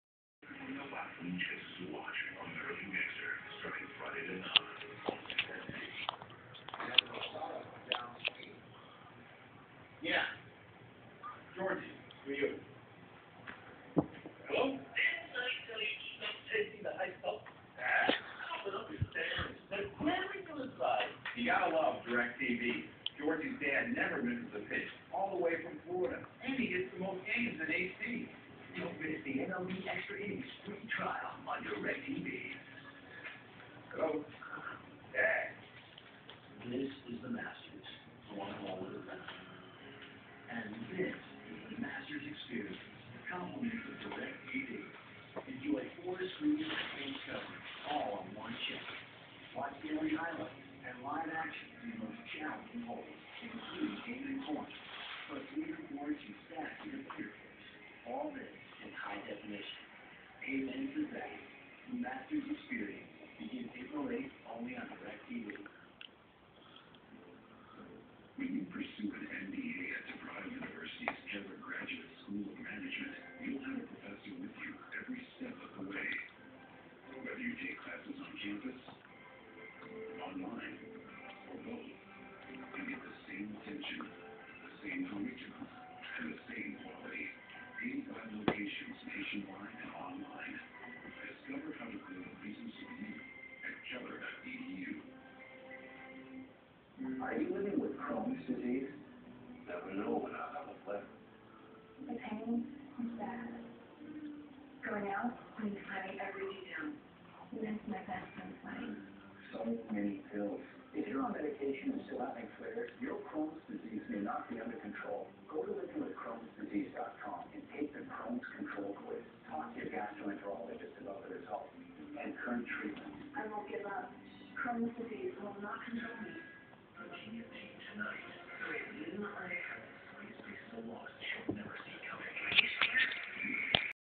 The One Sided Fight
Our cat Ben, when he was a kitten, diciding that my childhood stuffed animal Frank, who is a Tasmanian Devil, was his mortal enemy. This is just one of the many epic struggles they had.